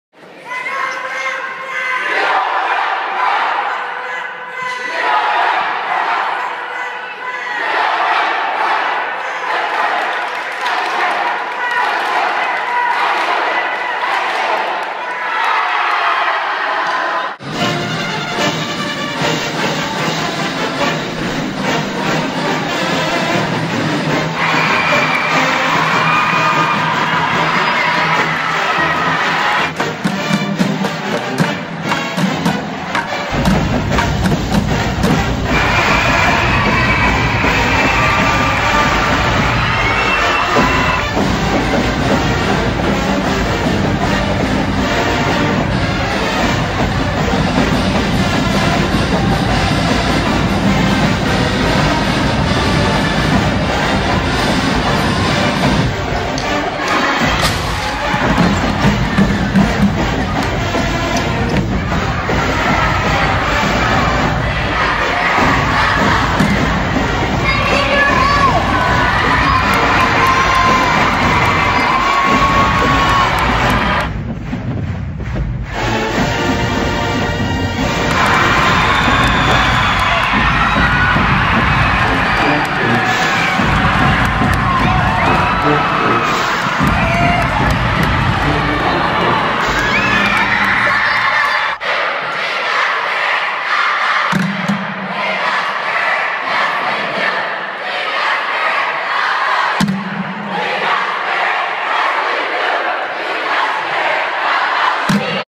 Middle School Pep Assembly
Highlights from the 2013 ASU Preparatory Academy Fall Middle School Pep Assembly